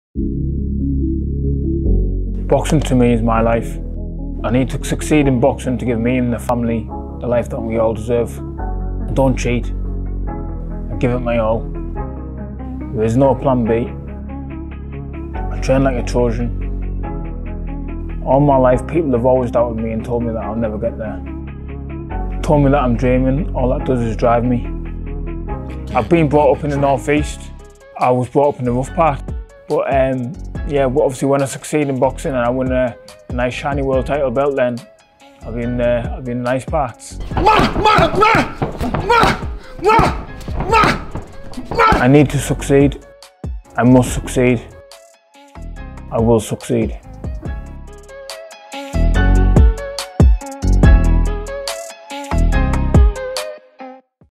"I NEED TO SUCCEED. I MUST SUCCEED. I WILL SUCCEED." | UNBEATEN TROY WILLIAMSON PROMO
Unbeaten super welterweight contender Troy Williamson takes on Harry Scarf live on BT Sport on Saturday night. Troy is quickly gaining a reputation as the most avoided man in the 154lbs division.